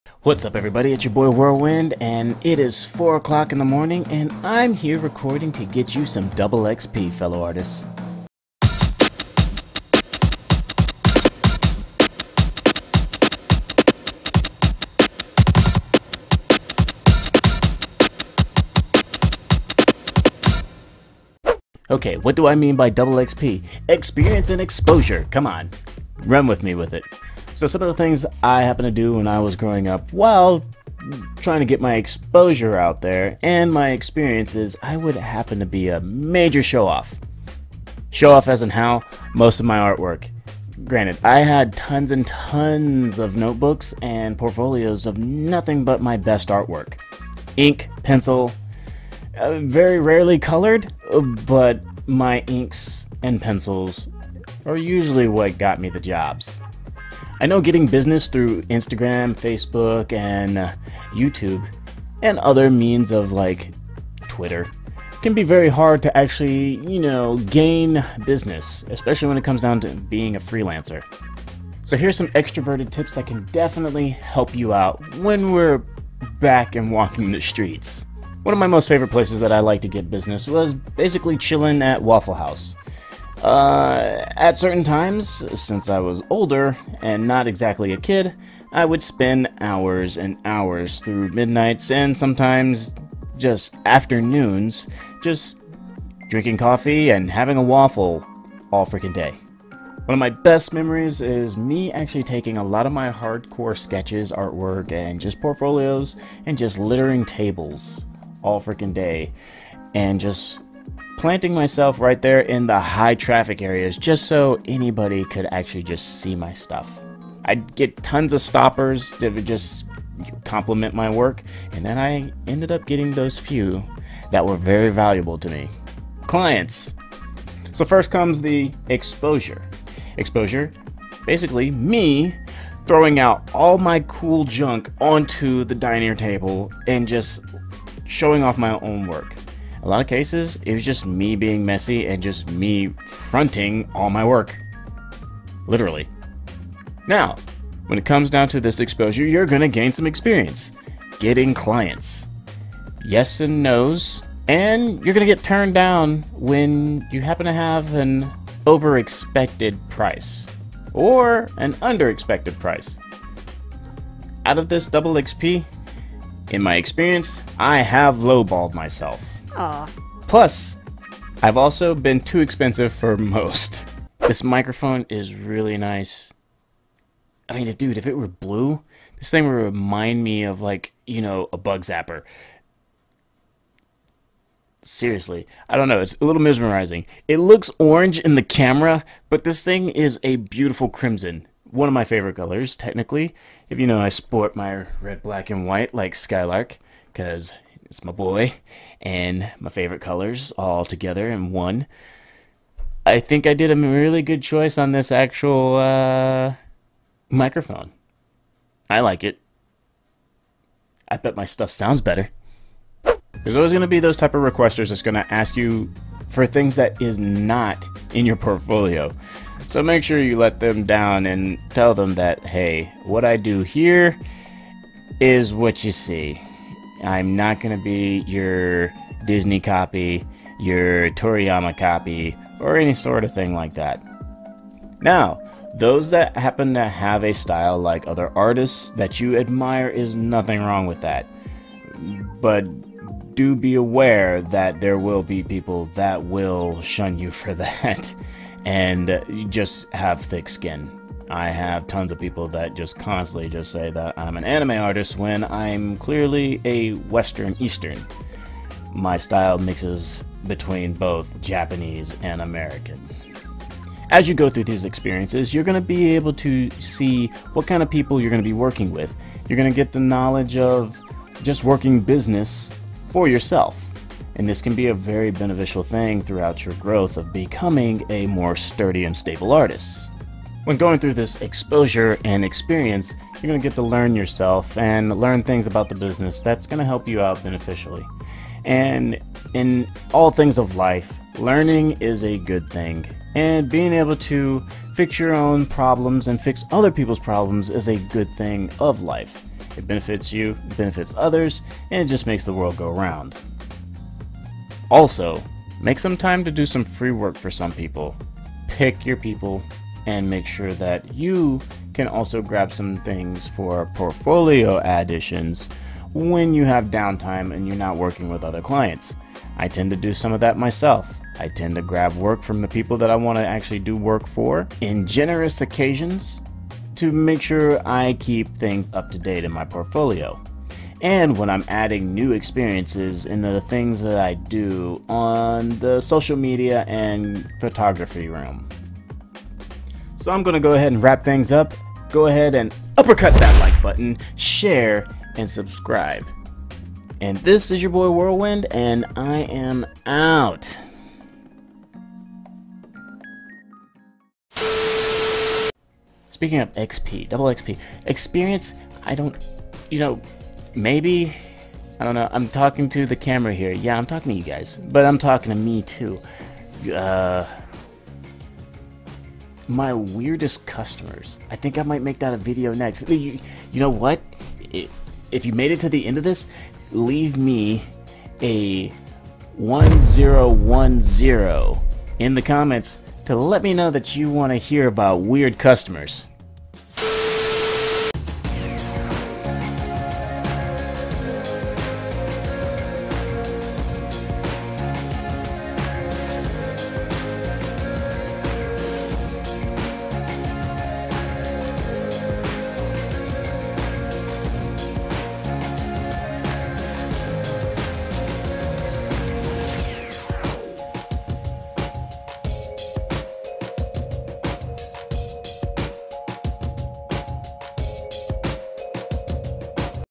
GETTING DOUBLE EXP IN THE ART HUSTLE
I don't mean to be a broken record... Though showing off your work in public areas is a great way to get local clients.

Many people need an artist for many different projects.
- Book Illustrations
- Character Design
- Comic Art
- Video Content
And more...

Hang out at your favorite restaurant, coffee shop, or book stores and layout your best work for display and watch for some of that attention and business come your way.

BUT. Let's wait for things to calm down a bit before planning your next Mocha and Chill.

[ :: All the hook ups SON!!! :: ]